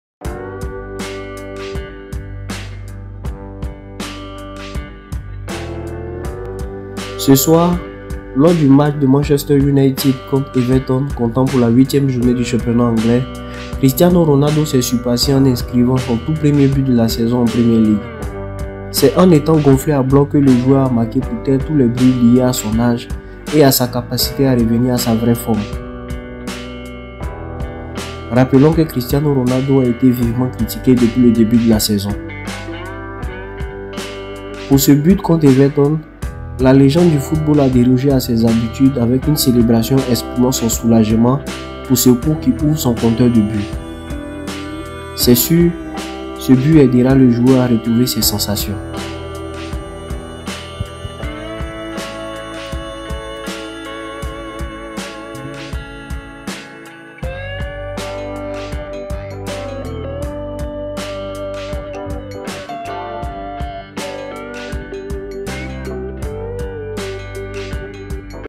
URGENT Ce que Cristiano Ronaldo a fait après son but de ce soir
Ce soir, lors du match de Manchester United contre Everton comptant pour la 8ème journée du championnat Anglais, Cristiano Ronaldo s'est surpassé en inscrivant son tout premier de la saison en Premier League...
Pour ce but contre Everton, la légende du football a dérogé à ses habitudes avec une célébration exprimant son soulagement pour ce coup qui ouvre son compteur de but.